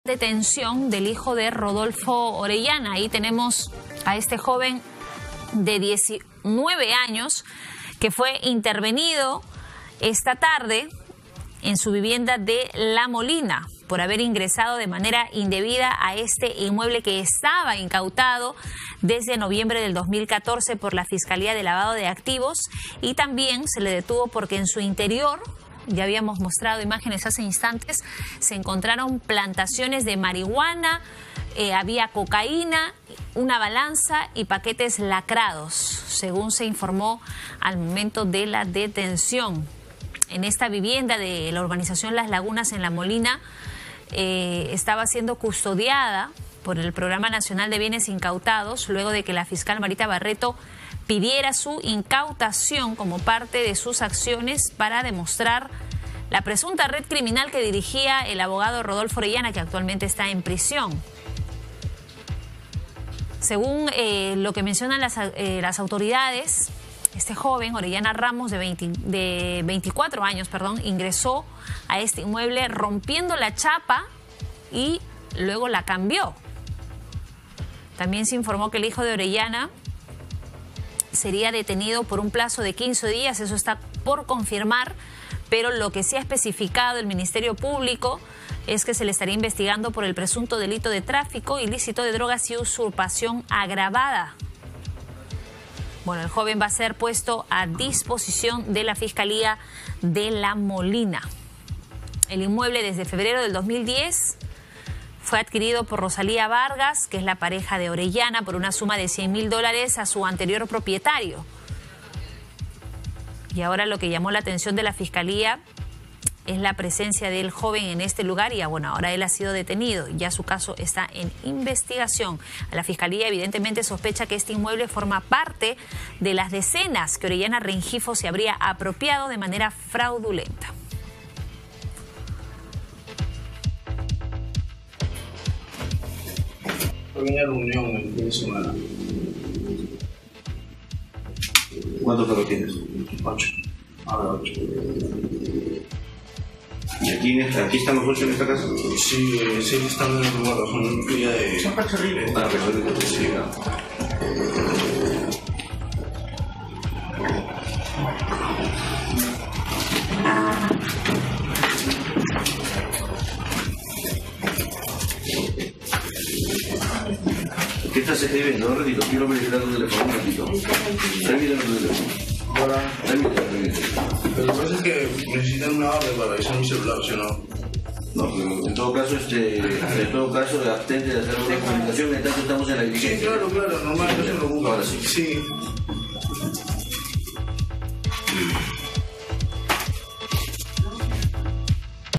Hijo de Rodolfo Orellana fue detenido en casa incautada por la Fiscalía
El Ministerio Público señaló que Rodrigo Orellana Ramos (19) fue detenido en flagrancia del presunto delito de usurpación agravada y tráfico ilícito de drogas.